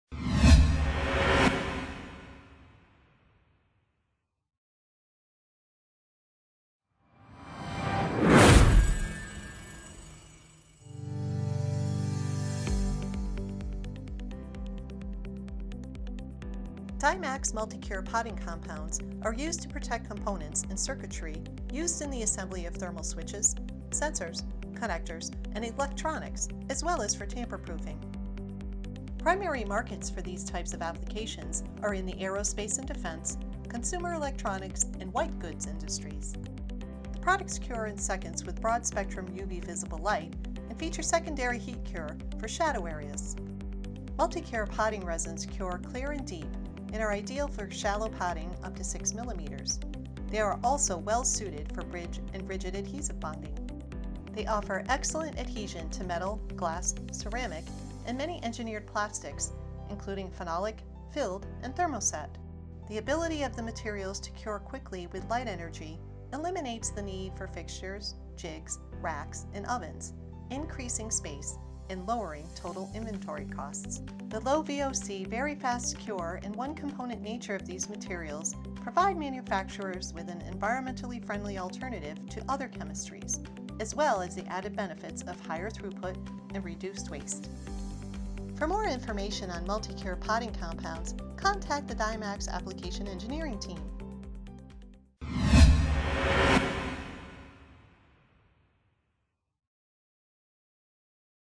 Encapsulados curados por luz para componentes electrónicos
Los materiales de encapsulado Dymax proporcionan uniones fuertes y translúcidas y son ideales para encapsular componentes electrónicos, conectores, interruptores térmicos y sensores, así como para protección contra manipulaciones. Ofrecen una alta adhesión a metal, vidrio, cerámica y muchos plásticos fenólicos, rellenos y termoendurecidos.

Contacto